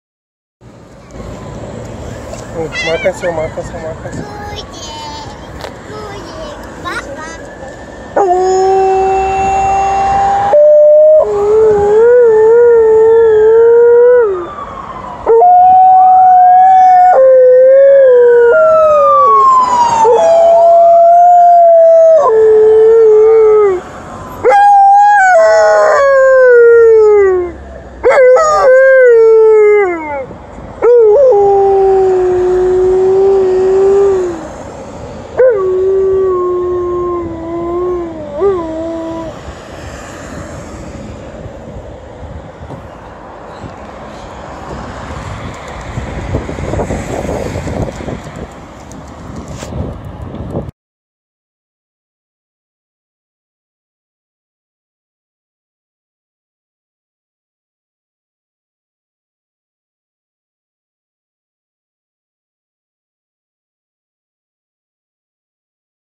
Alaskan Malamute is howling to the ambulance
Liza, the Alaskan Malamute, is howling to the ambulance on an emergency run.